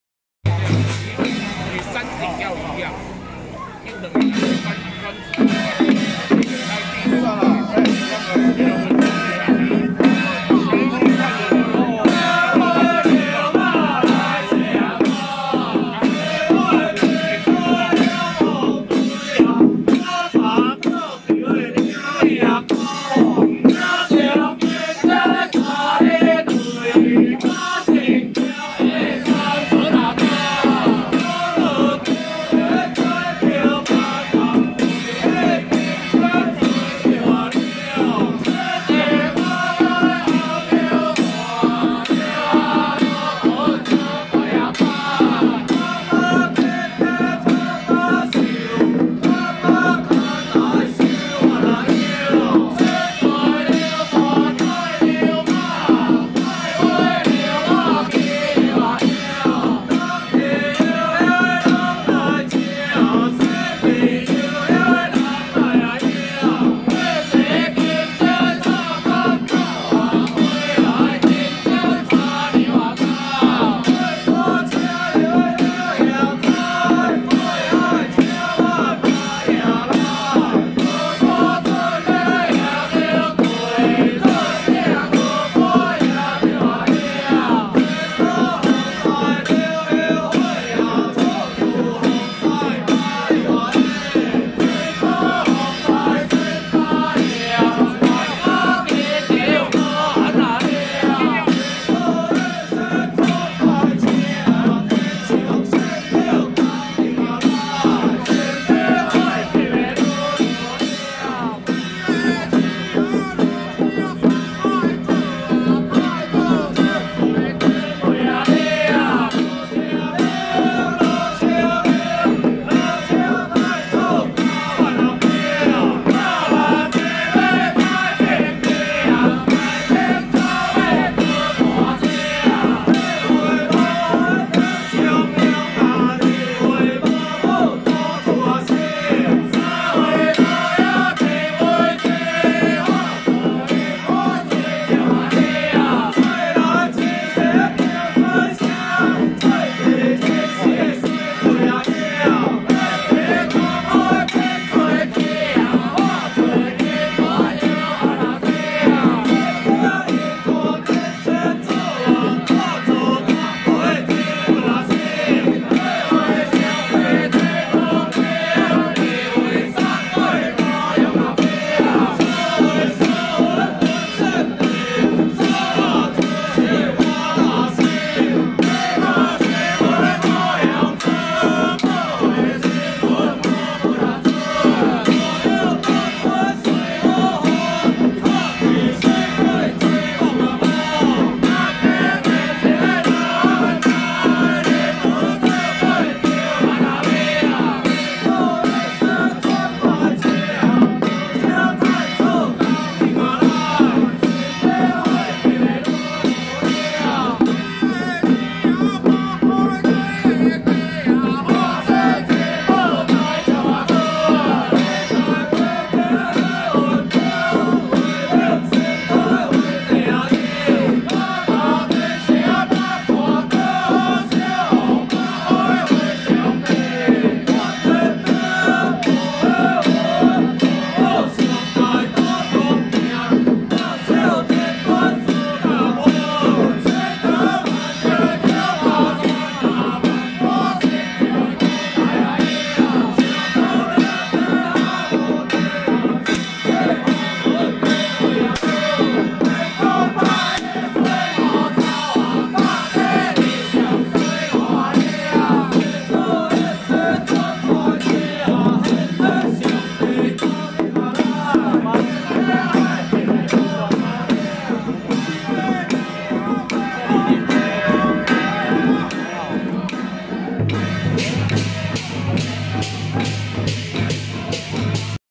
【台北尊澤堂協興壇-小法團】~109板橋龍德宮北極玄天上帝三年圓科南巡謁祖進香回駕遶境
台北尊澤堂協興壇-小法團